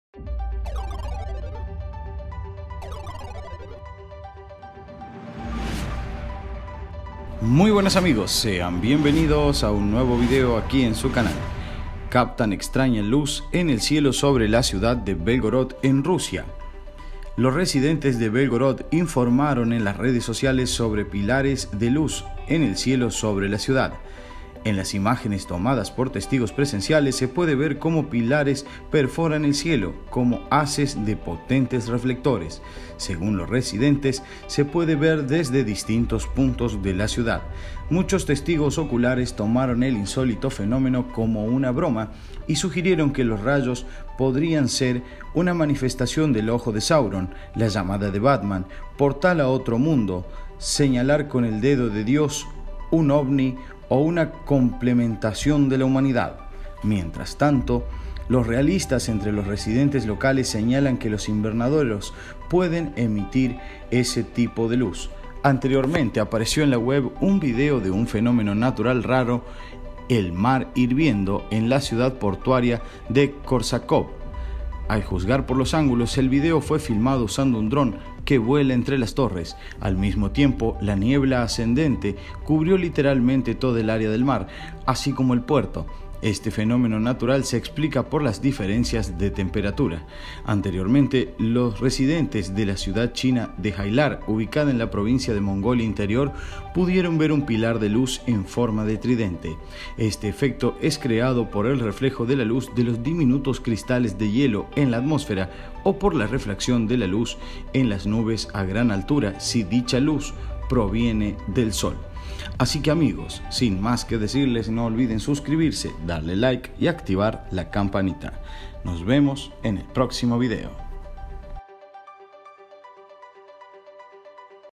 URGENTE: ESTA LUZ MISTERIOSA APARECIO EN EL CIELO EN RUSIA, QUE SIGNIFICA?
Toda la Informacion TENDENCIA DEL MUNDO, RELEVANTE E IMPORTANTE DEL MUNDO  !!